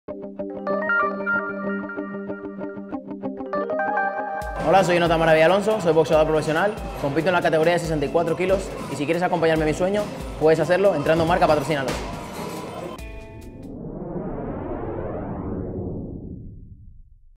Presentación del proyecto de Jonathan 'Maravilla' Alonso
El boxeador hispano-dominicano Jonathan 'Maravilla' Alonso te anima a sumarte a su ambicioso reto en MARCA-Patrocínalos. Apóyale y consigue fantásticas recompensas de forma directa.